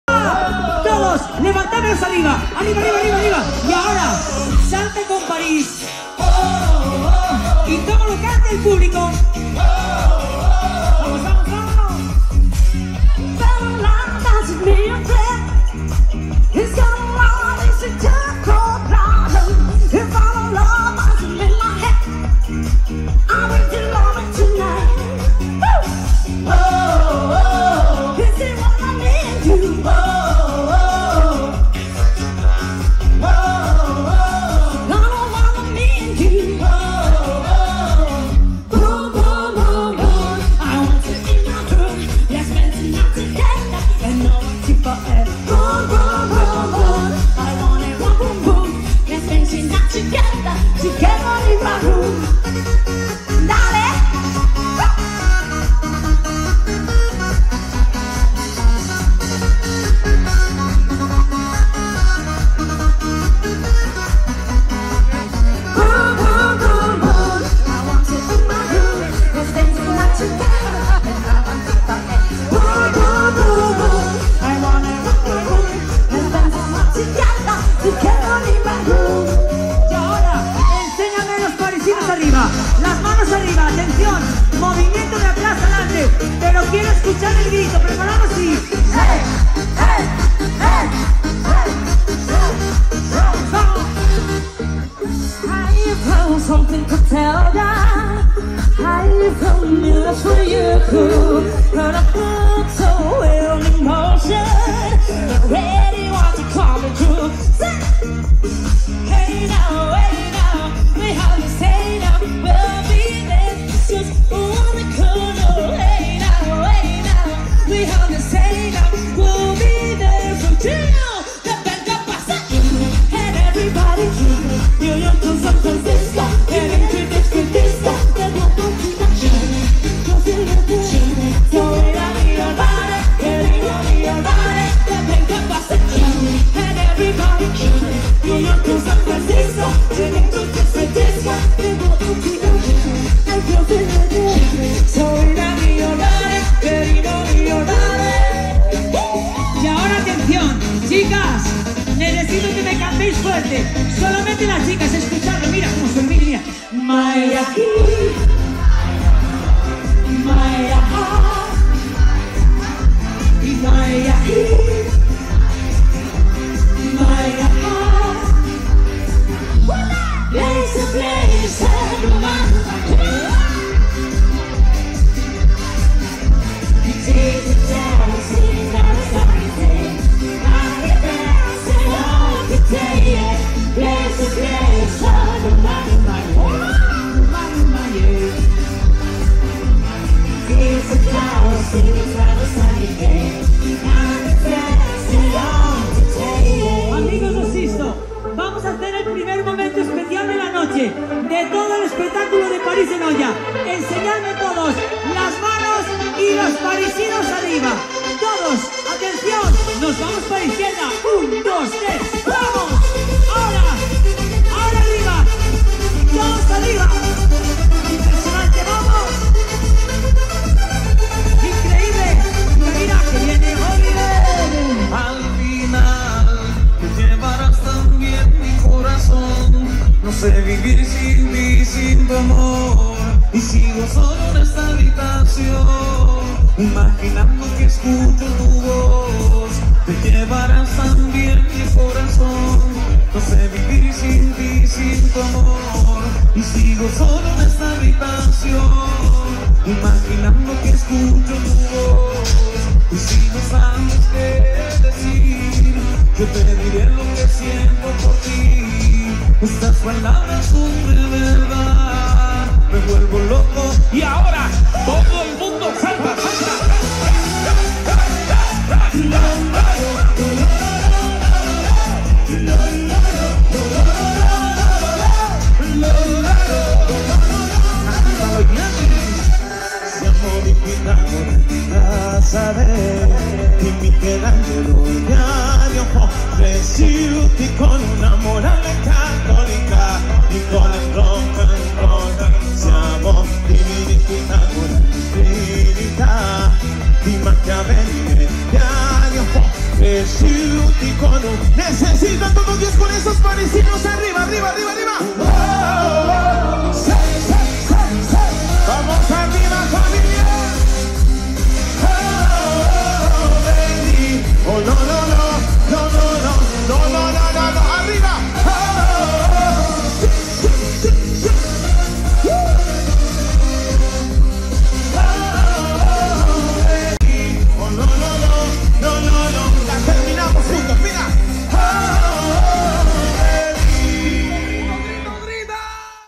Orquesta Paris de Noia 2023/2024, Popurri Dance
O Sixto, Xanza, Valga, Pontevedra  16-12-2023